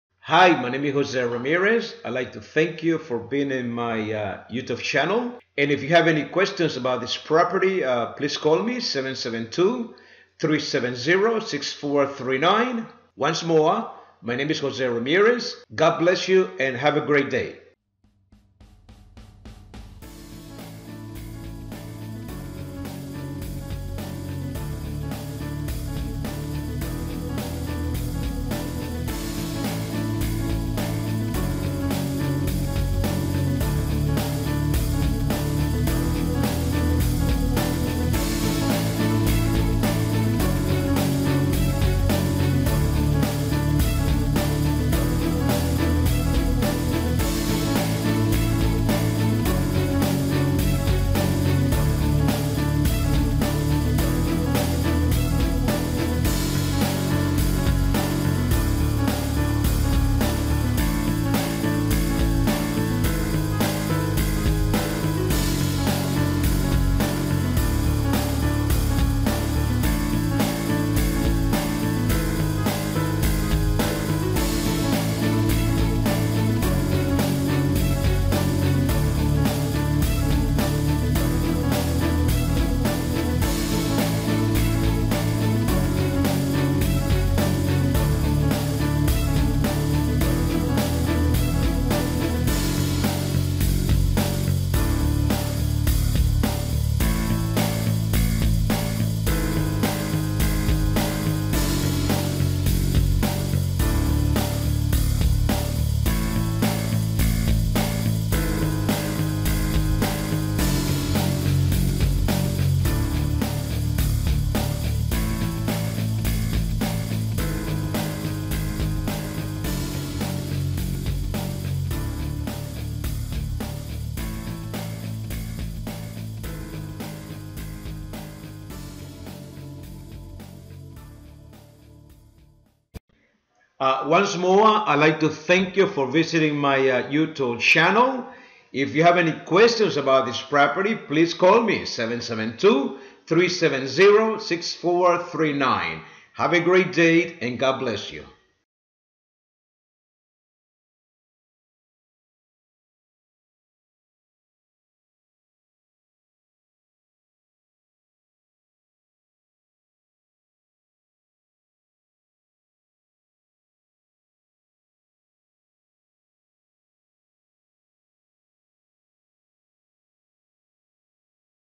land for sale punta gorda fl
land for sale punta gorda fl - Fantastic Piece Of Land $32,900 - Huge And Waiting For Someone To Build A Dream House - Won't Last. This Includes 5 Lots Totaling Over Half Acre. You Can Split Them Out For 2 Building Lots. Located At The Corner Of Broad Point And Citrus. Close To I-75 Located In Harbour Heights. Community Has A Boat Ramp For Quick Access To The Peace River And Gulf.

WHAT IS YOUR FLORIDA HOME WORTH NOW?  GET YOUR ANSWER IN 15 SECONDS!